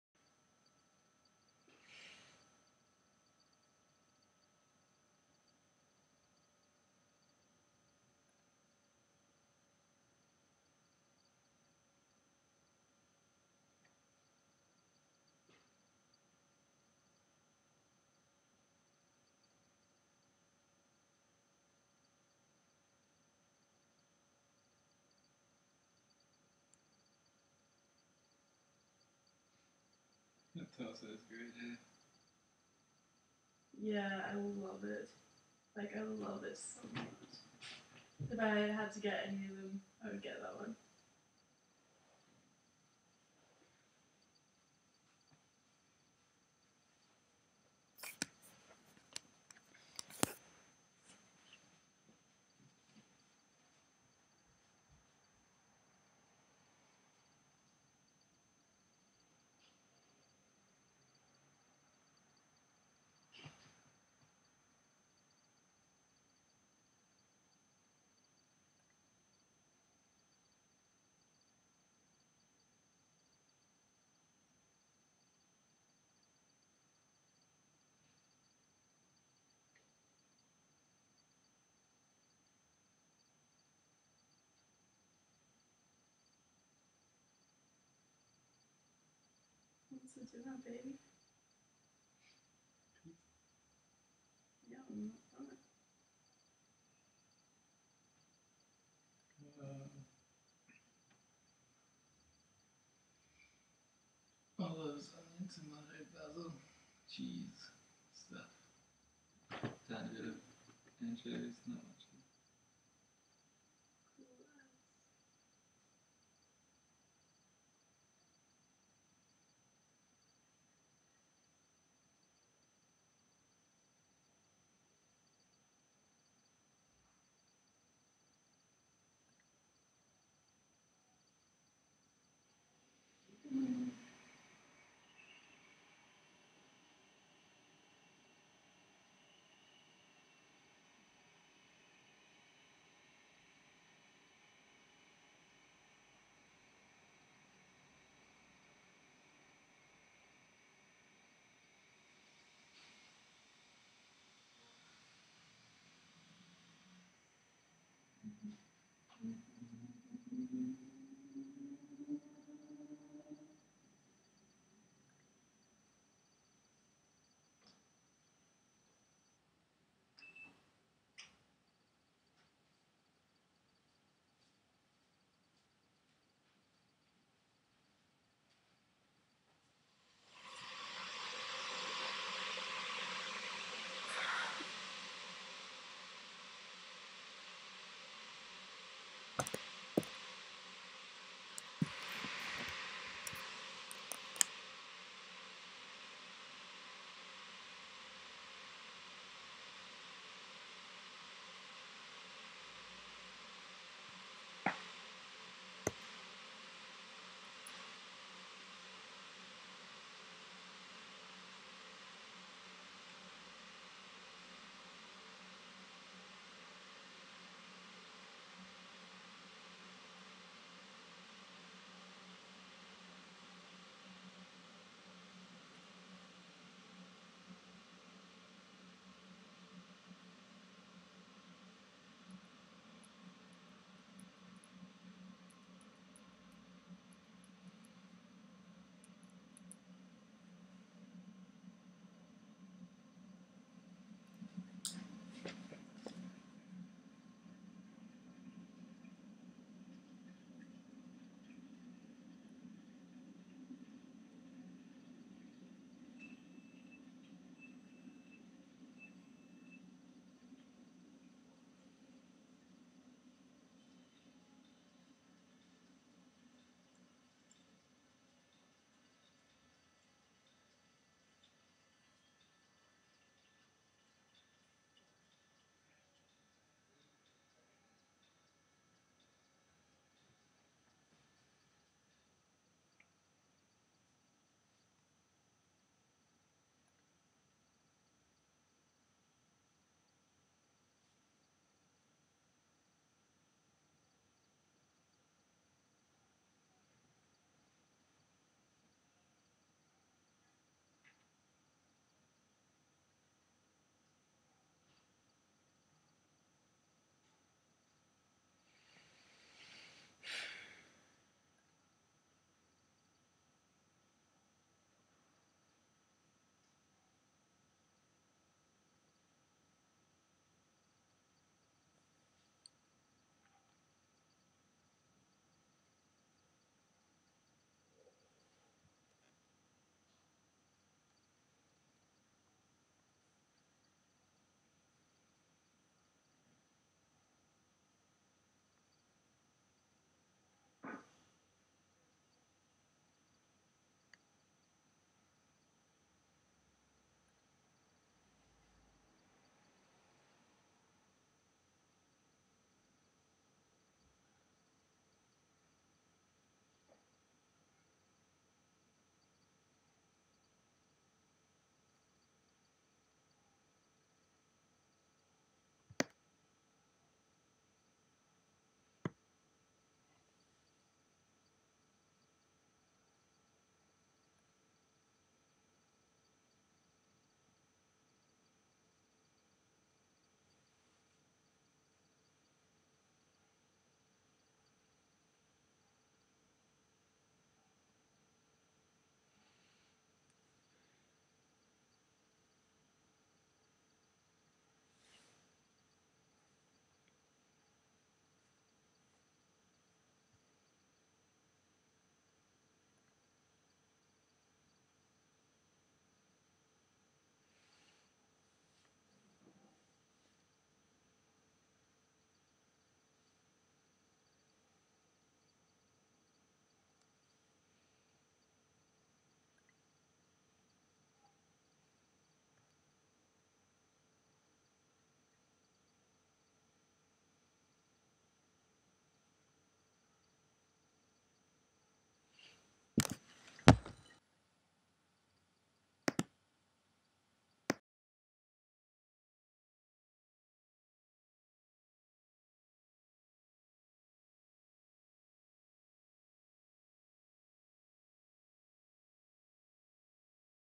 29/3/15
This is part of a video project in which I film myself whenever I have my laptop open and put it on YouTube totally unedited.